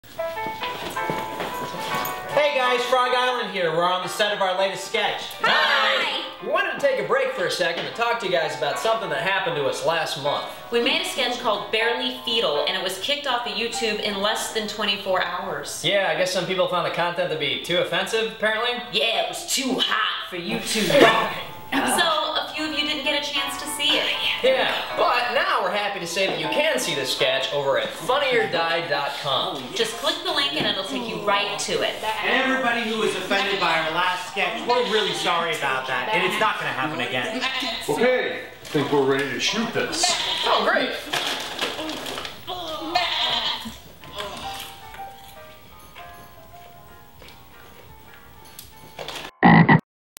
"Barely Fetal" promo
Frog Island apologizes for their last sketch, "Barely Fetal"